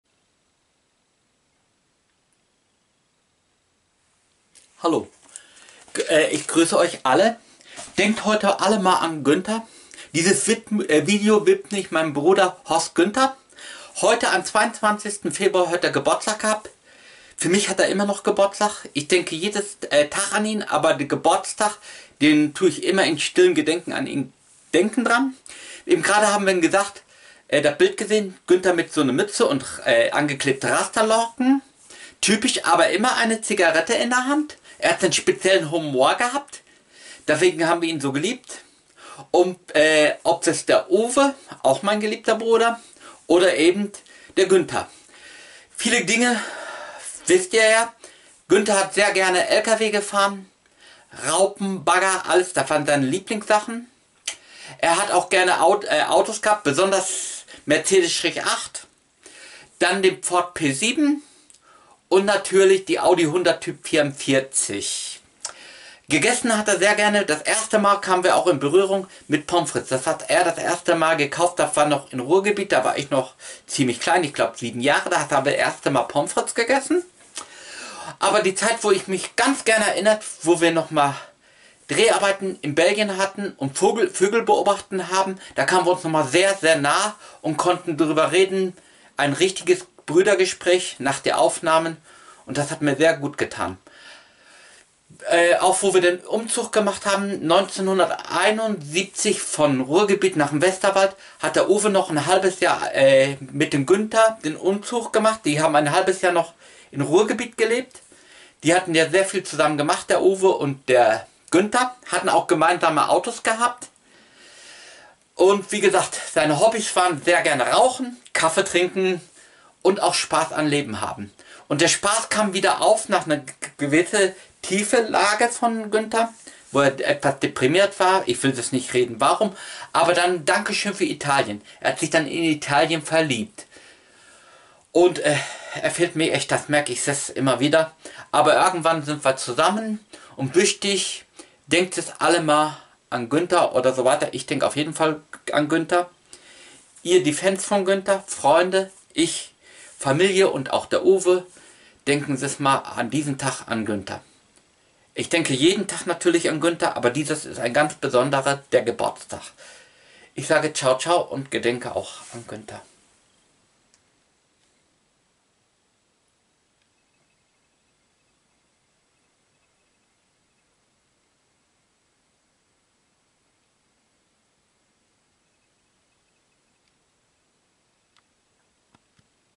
Horst- Günters Ehrentag
Am heutigen Tag  wäre Horst- Günter 66 Jahre alt geworden.